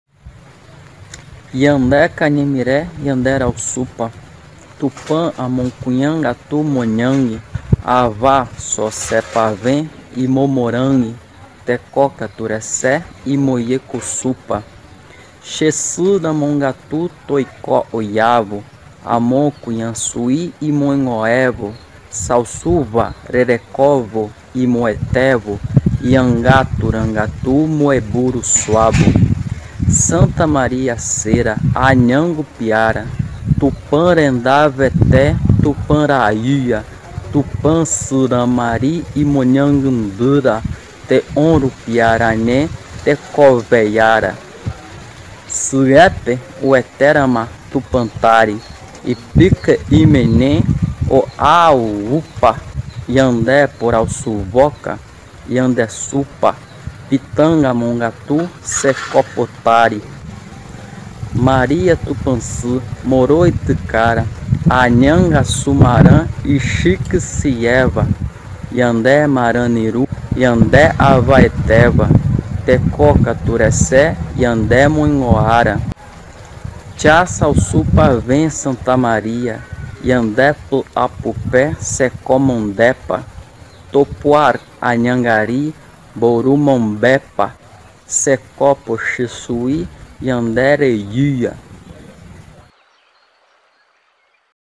Poema de Anchieta QUERENDO O ALTO DEUS (Tupi Antigo)
Extraído do Livro: POEMAS Lírica Portuguesa e Tupi (José de Anchieta) - Organização: Eduardo de Almeida Navarro. Editora Martins Fontes. 2004. Pág 86-89.